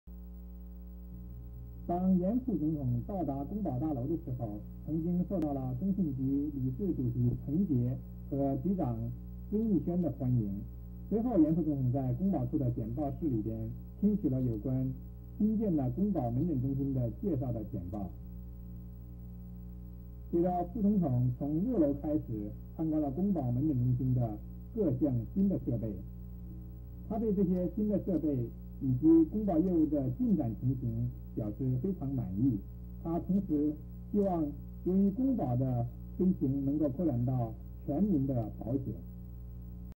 嚴副總統參觀北市公保門診大廈
(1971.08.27)當嚴副總統到達青島路公保大樓的時候，受到多人的歡迎，並且聽取了有關醫院的各項簡報，隨後參觀公保門診大廈裡面的各項新的醫療設備，他對這些新設備的使用情形表示非常滿意，希望這是全民的福祉. (配音)